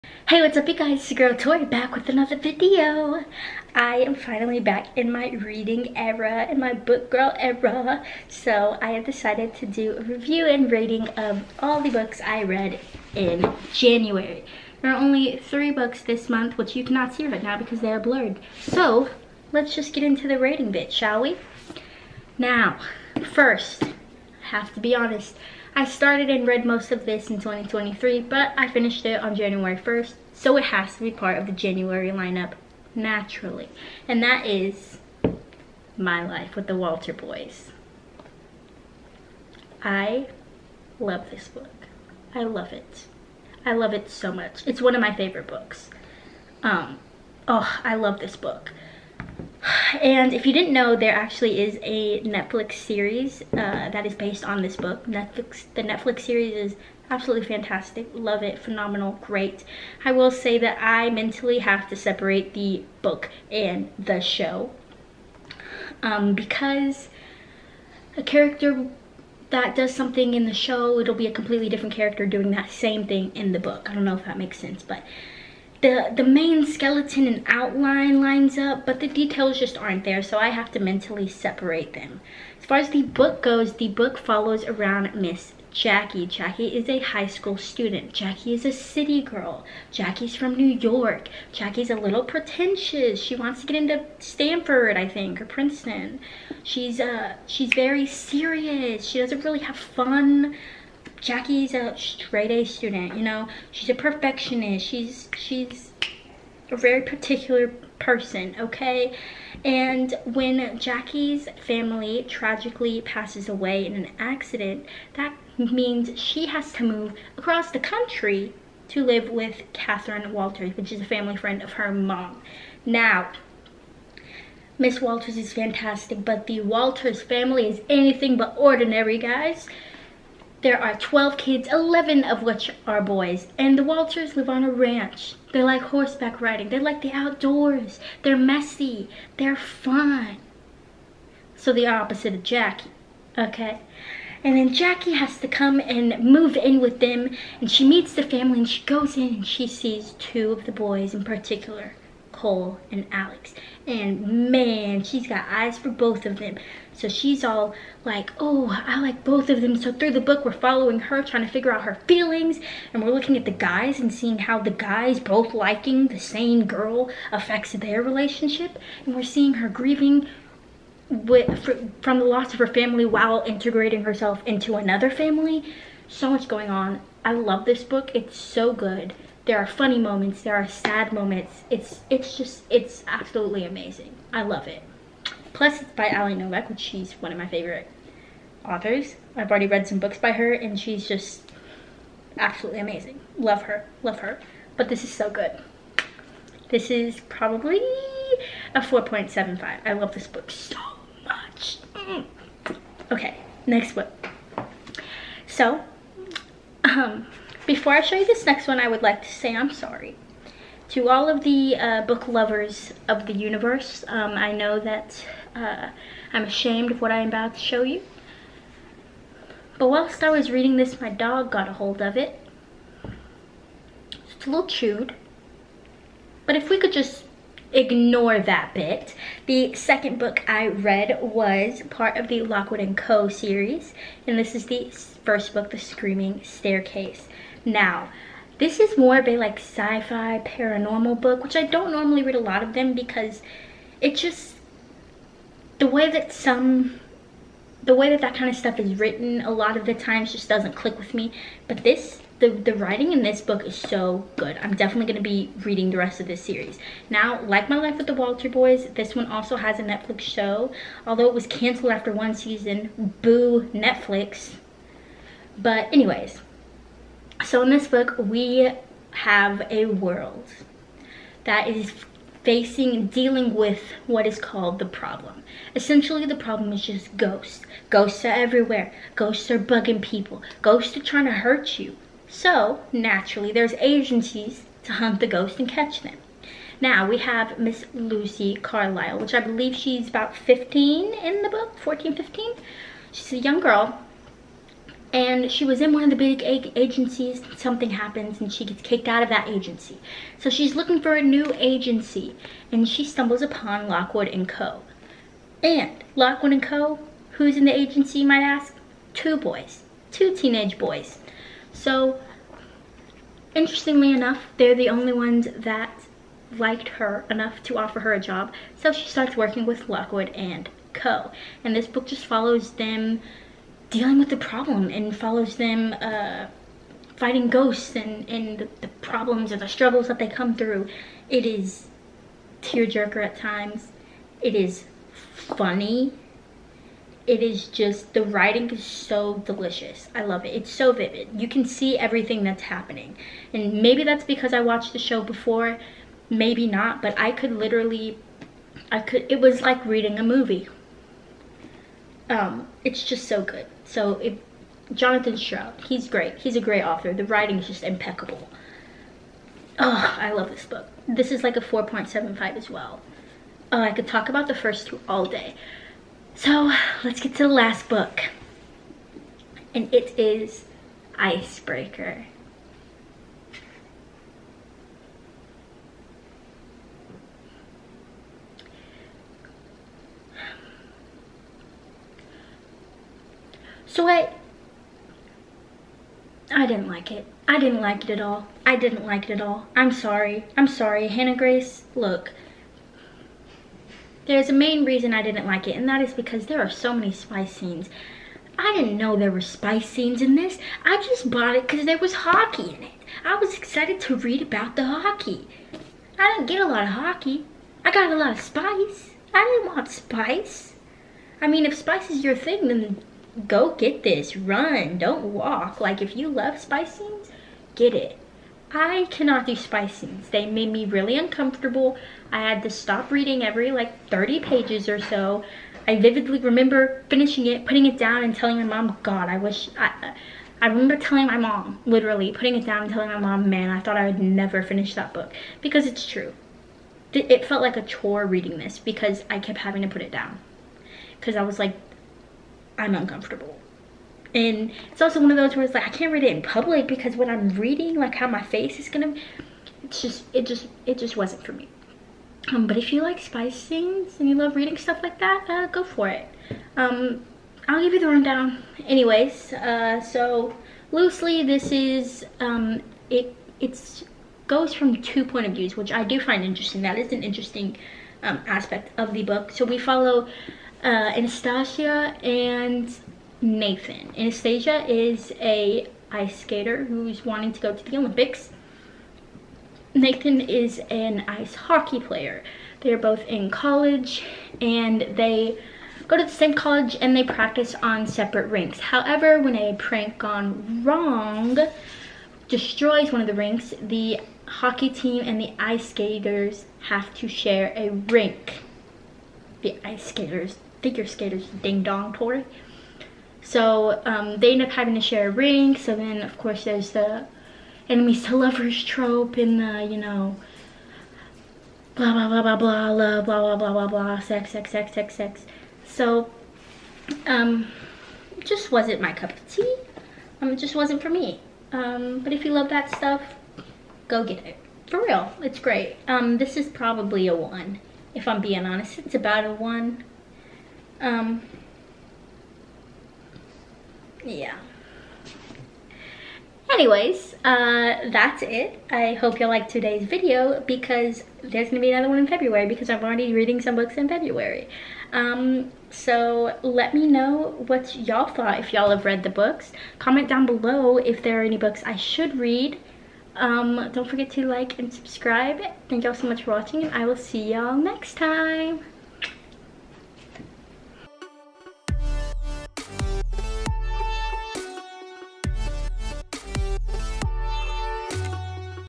Reviewing and Rating the books I Read in January
Back in my book girl era! Hope yall like this viedo because more book videos are coming!

 Love yall!!!